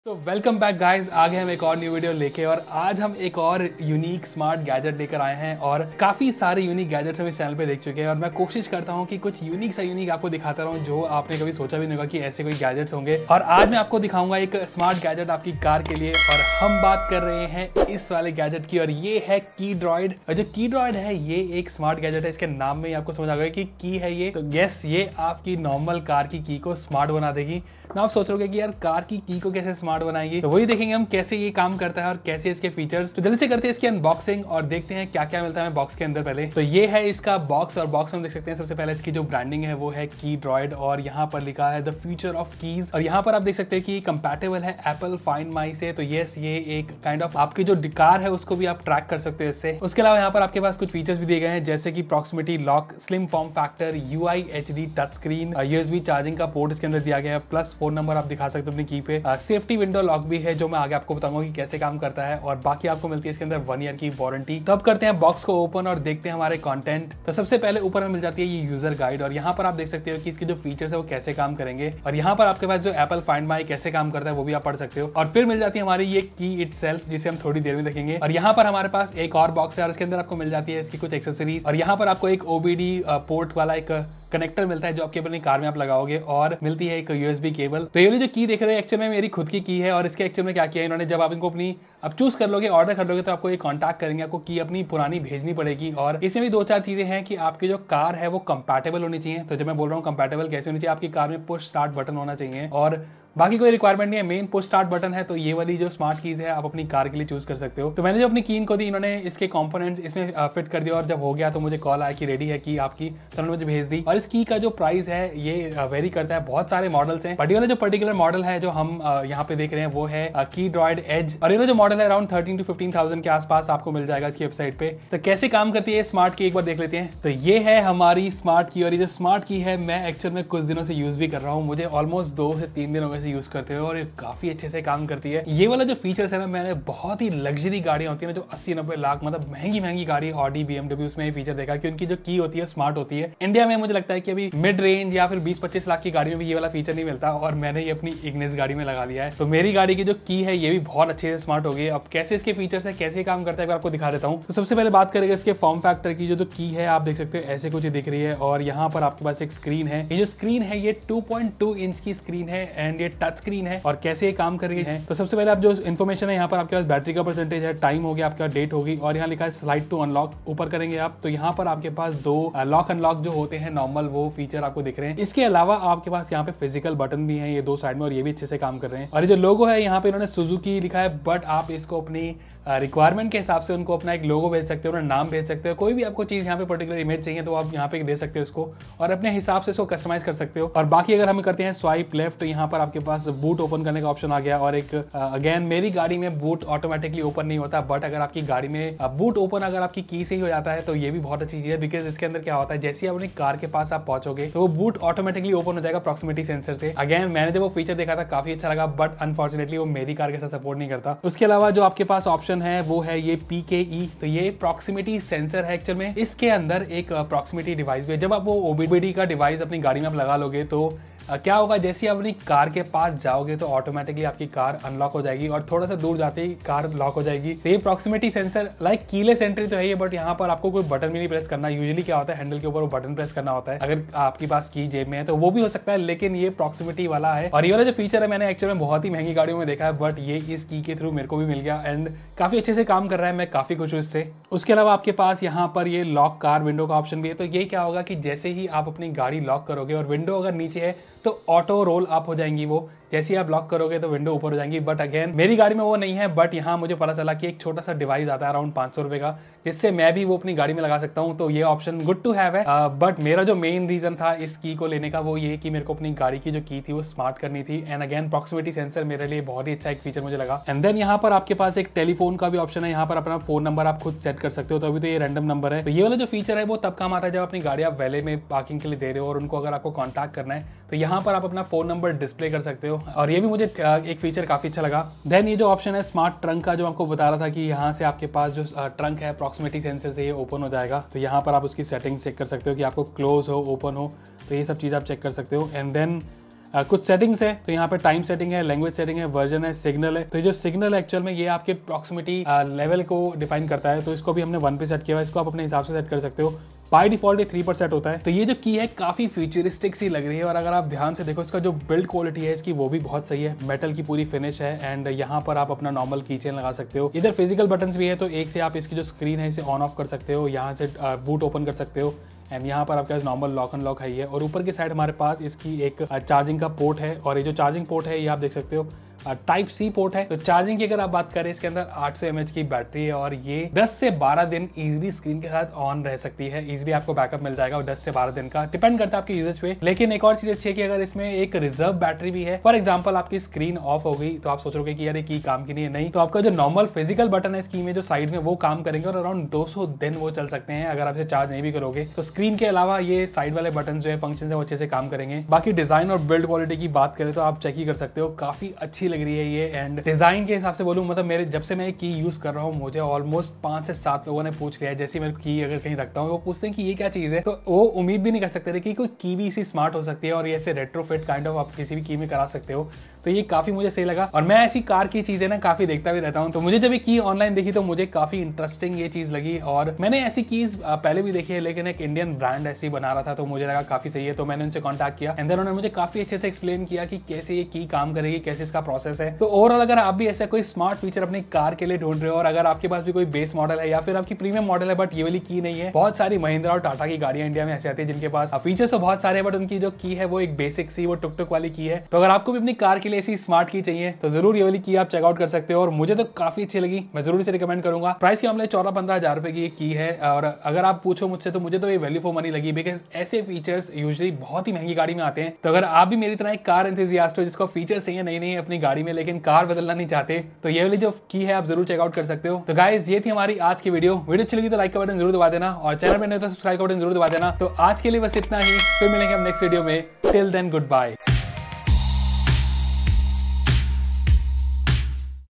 I Converted My Car’s Key into a SMART KEY - ⚡️ Keydroid Edge Smart Key - MINDBLOWING 🤯🤯
I Converted My Car’s key into a SMART KEY -  Smart Key - Car Gadgets - Car Accessories - Unique Car Gadgets - Best Car Gadgets - Best Car Gadgets 2023!

Today we are going to show you a smart key for your car. And this key will give you features of a high end cars. With this smart key you can do lot of things and in this video we will do the unboxing and show you the features of this key.

If you have any questions or queries drop them in comments section below.